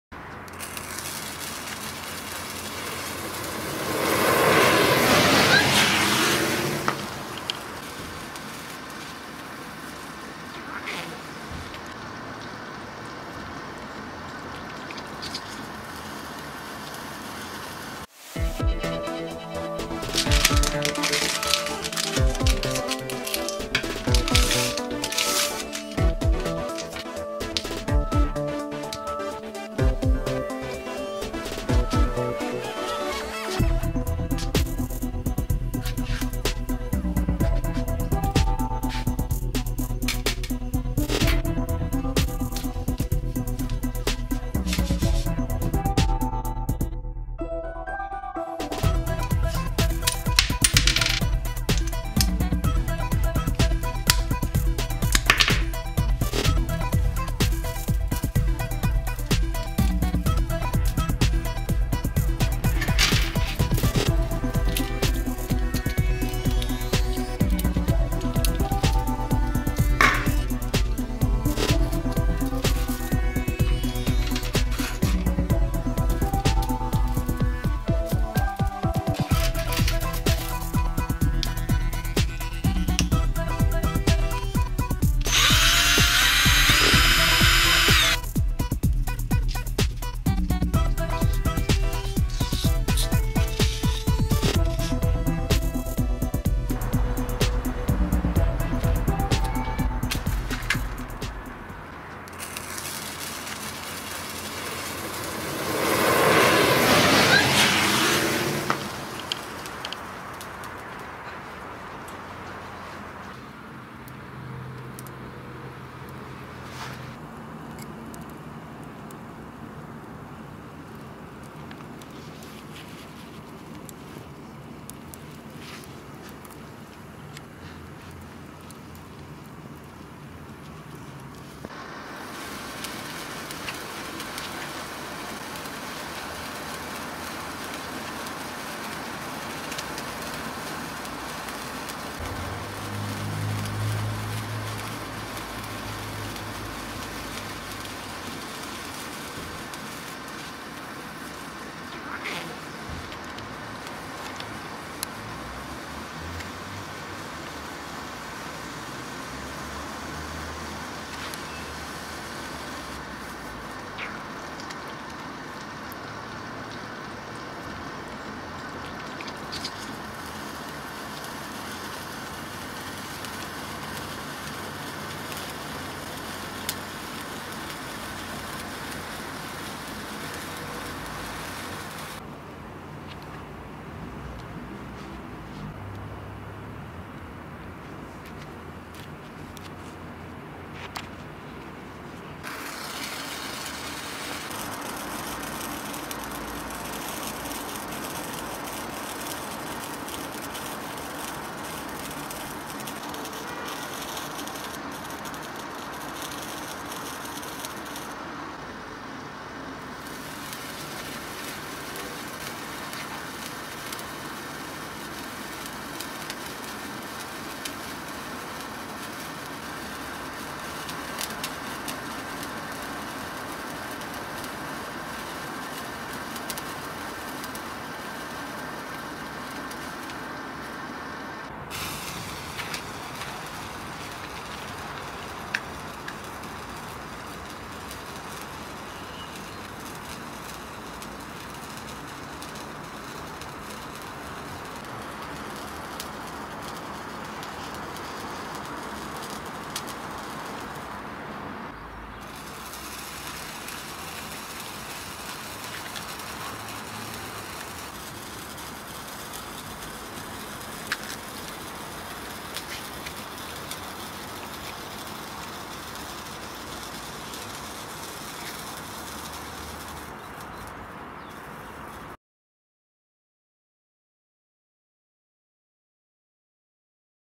Sparklers VS Fidget Spinner, Egg, Fruits & Vegetables
Watch this awesome video to see how sparklers react with different types of objects.